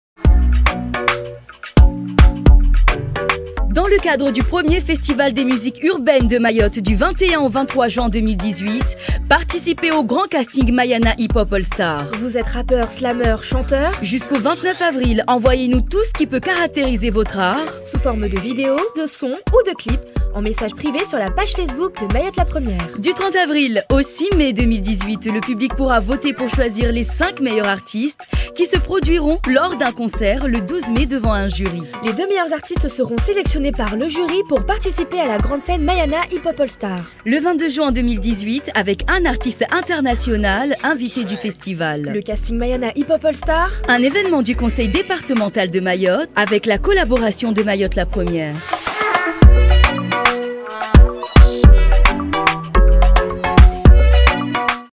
≡ CASTING ● MAYANA HIP HOP ALL STARS
Etes -vous musiciens, rappeurs, slameurs, chanteurs ou youtubeurs ?
Envoyez-nous dès maintenant vos clips en message privé sur Mayotte la 1ère!